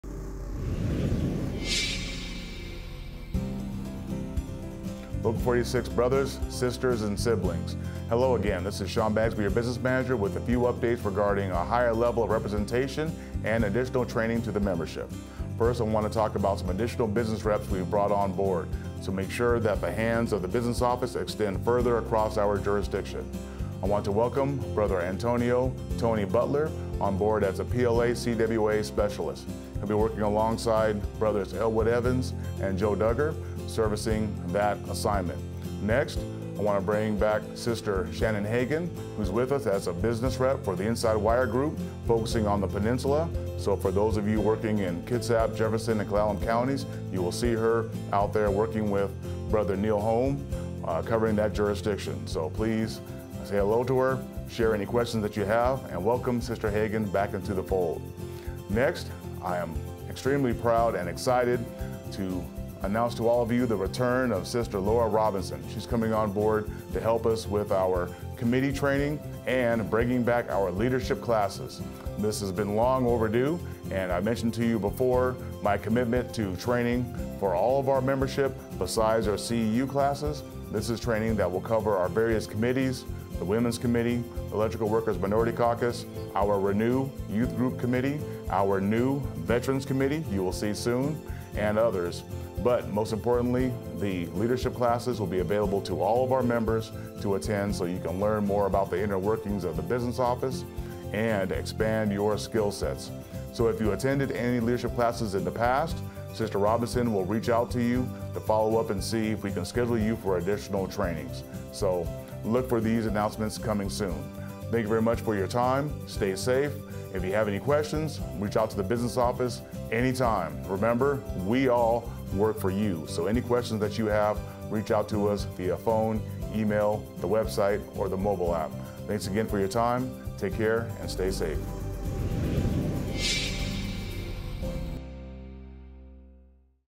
IBEW Local 46 Business Manager Sean Bagsby introduces some of your new Reps.
Sean Bagsby introduces some of your new Reps at Local 46 and announces the return of our Leadership classes.